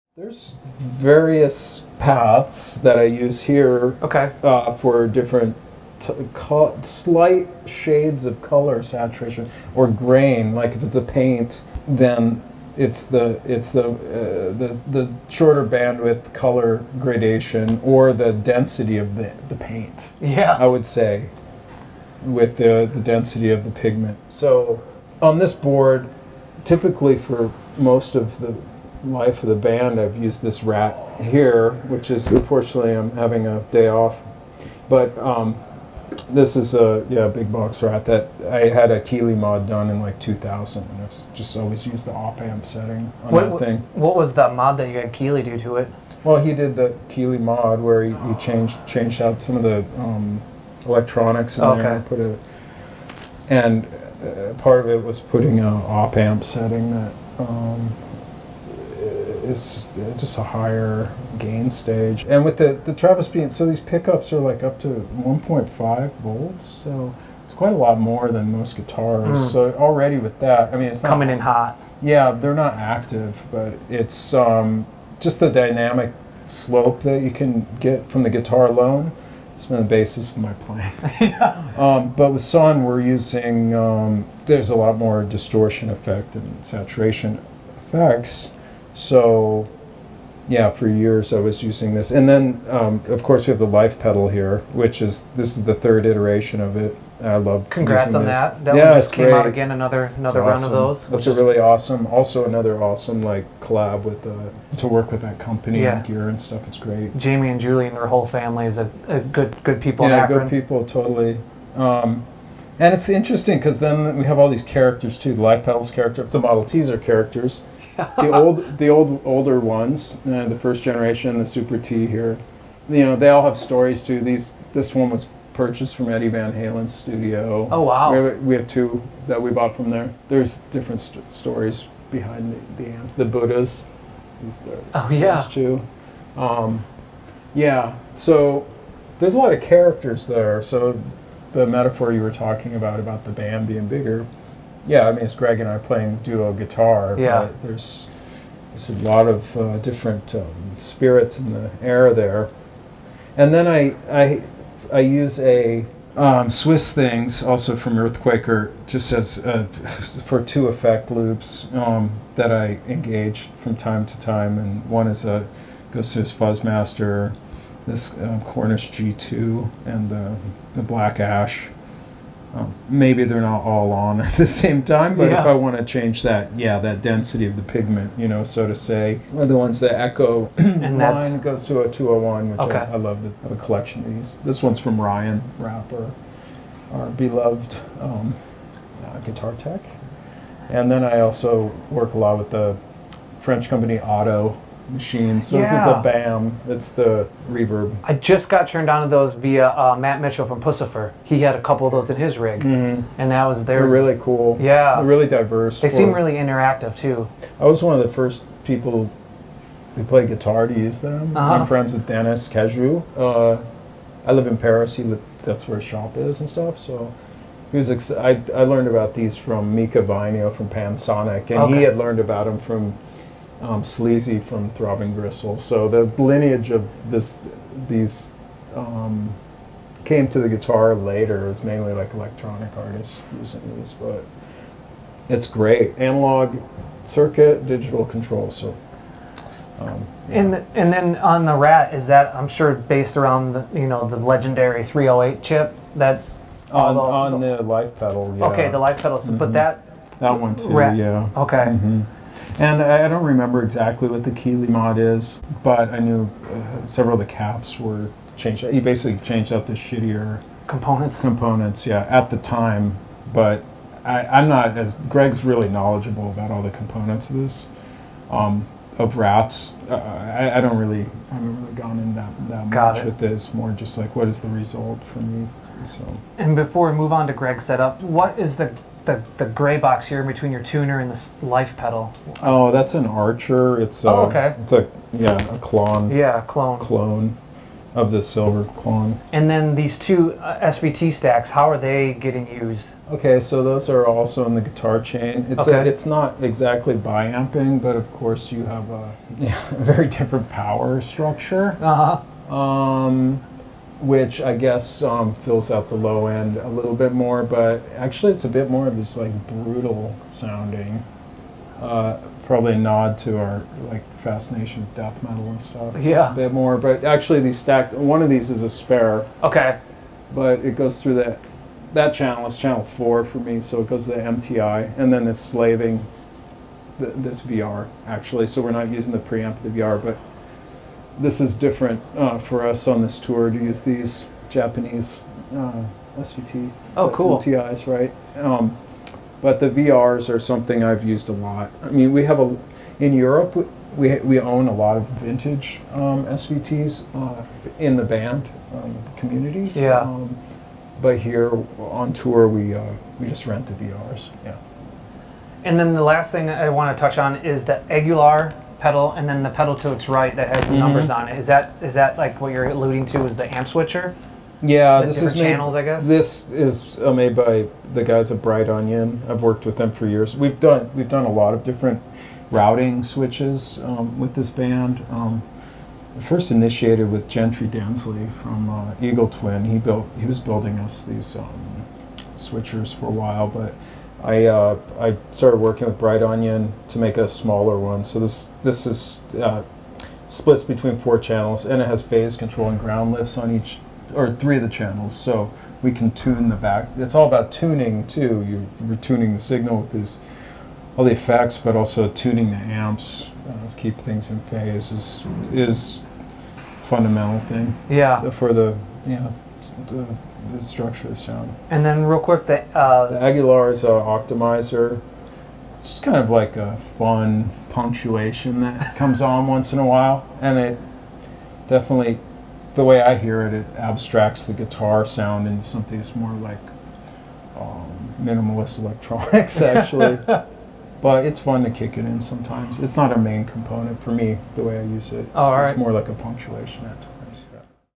Stephen O'Malley's Sunn O))) Pedalboard
Sunn O))) cofounder Stephen O'Malley walks us through the levels and stages of filth and fury at his fet.